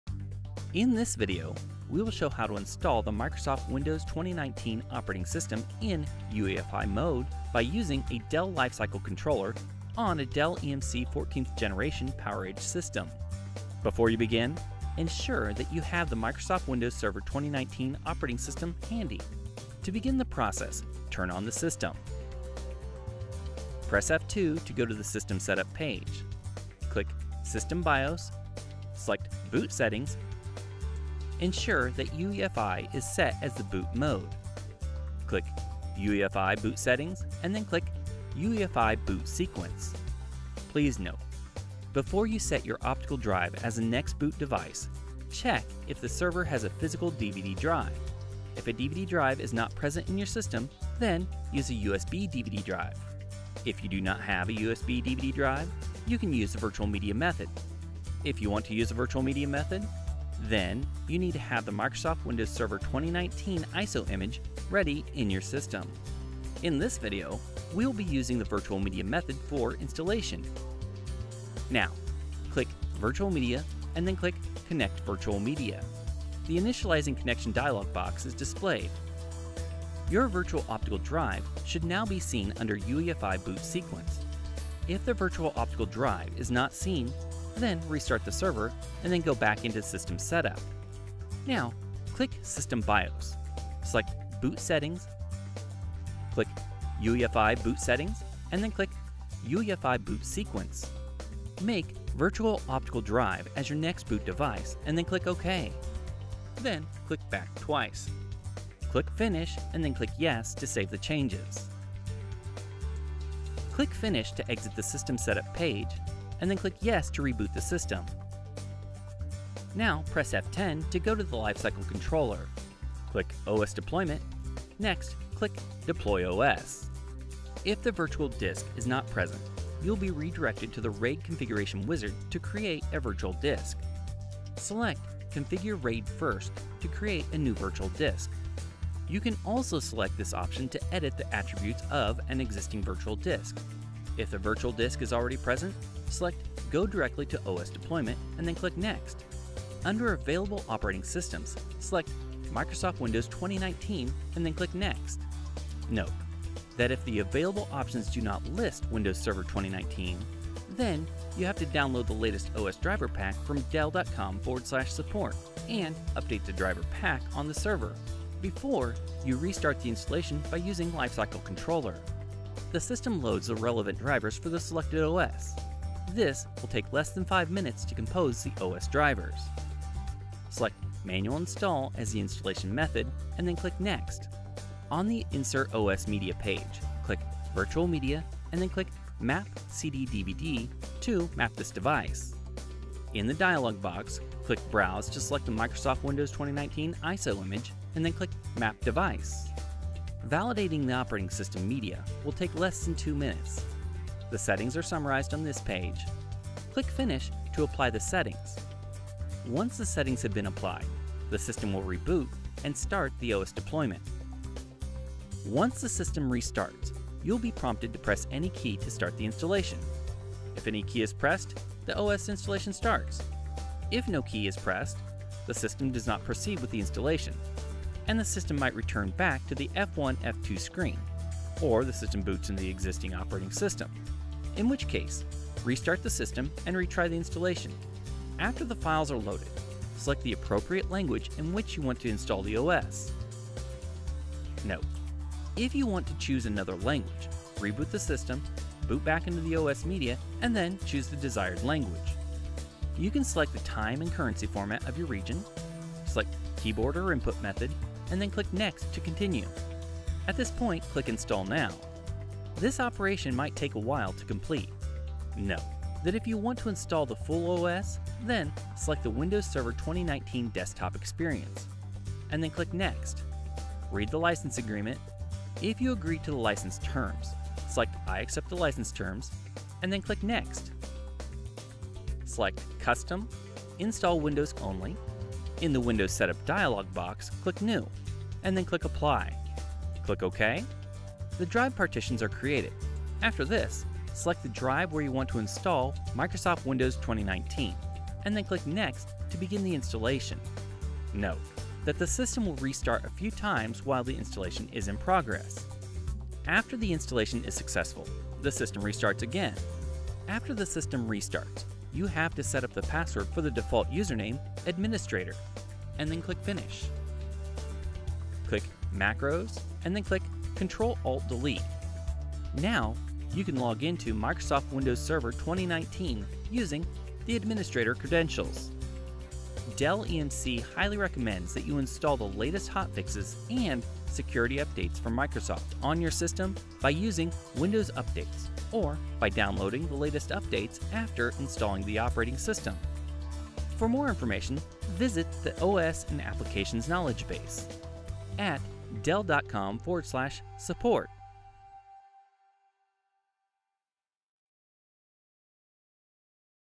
Install Windows Server 2019 in UEFI Mode Using Lifecycle Controller on PowerEdge Servers
Demonstrates how to install the Microsoft Windows Server 2019 operating system in UEFI mode by using Dell Lifecycle Controller on Dell PowerEdge servers. for model specific support.